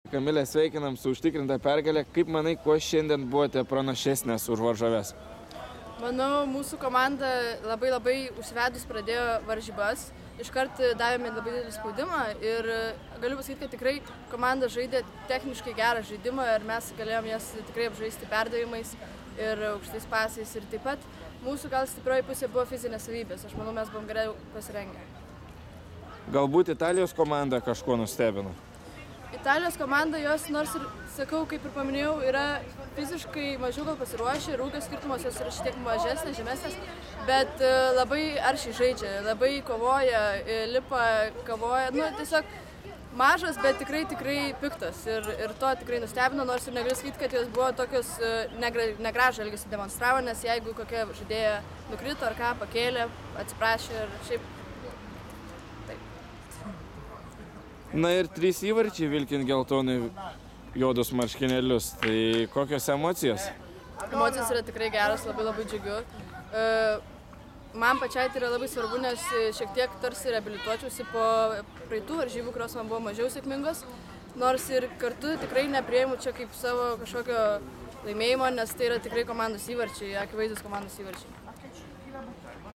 K.Vaičiulaitytė: itales pranokome fizinėmis savybėmis
"Gintros-Universiteto" komandos puolėjos Kamilės Vaičiulaitytės komentaras po pergalingų rungtynių 5:1 prieš Romos "Lazio Rapples" komandą. Kamilė šiose rungtynėse pasižymėjo 3 kartus.